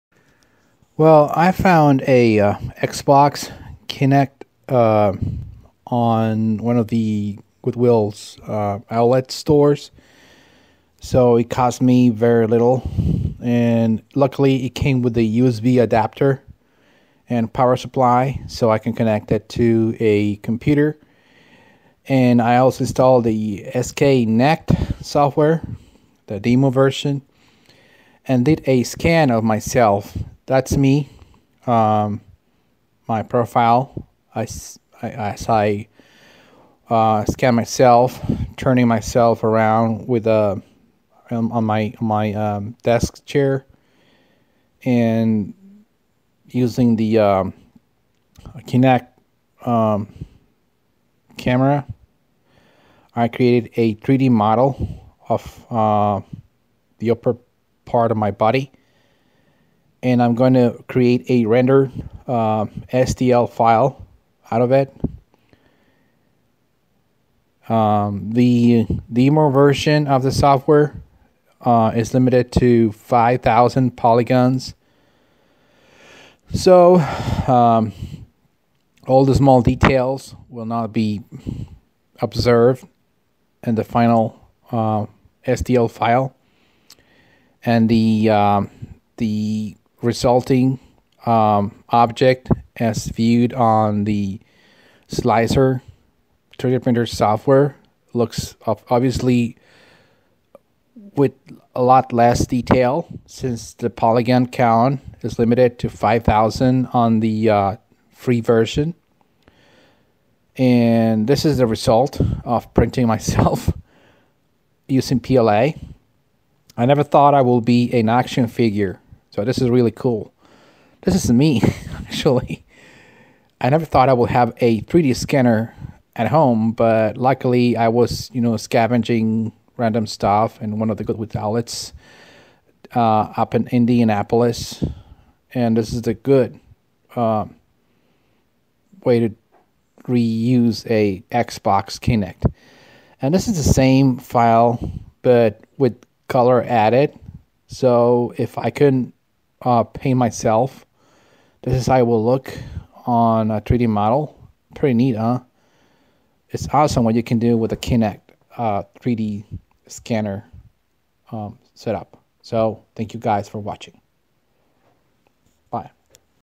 kinectScanner
I found a xbox 360 kinect system with the USB adapter at a Goodwill outlet store for literally pennies! - Well I installed the Windows SDK and VC++ redist 2015 and a free version of a 3D scanner software and voila!  I've got myself an action figure of myself - well, sort of ;)!